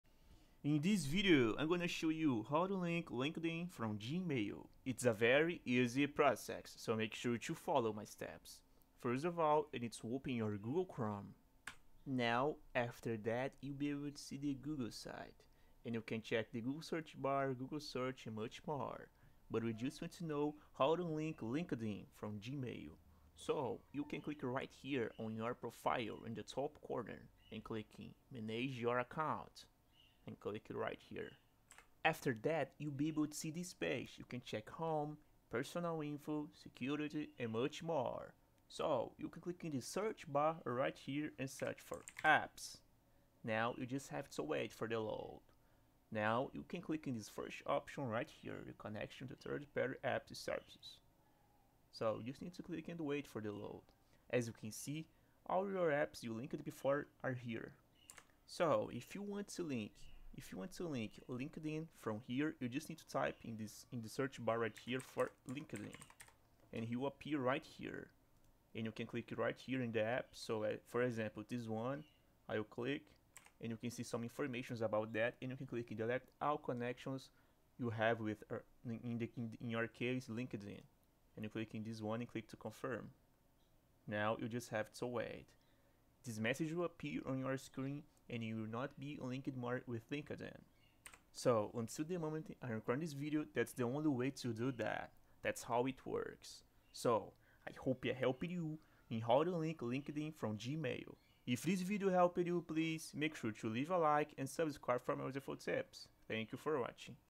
How To Unlink Linkedin from Gmail (Very Easy!)
Welcome to the video: How To Unlink Linkedin from Gmail

In this video I will clear your doubts about how to unlink linkedin from gmail, and whether or not it is possible to do this.